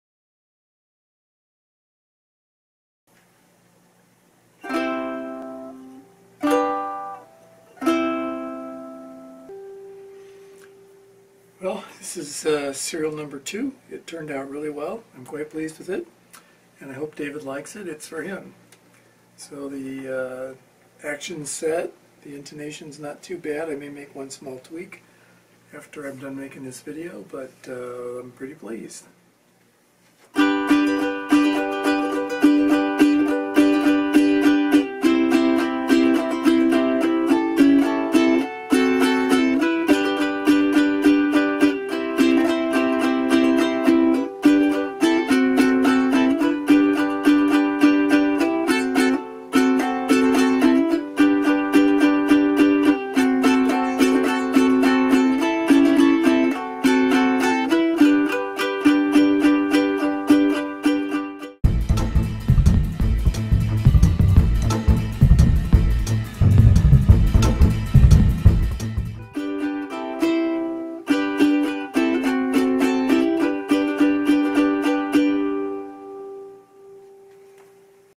SN 002 Heavy Log ready to ship
I finished Ukulele SN 002 and it is ready to box up and send to Texas.

It turned out quite well.  No on to SN 003 and SN 004!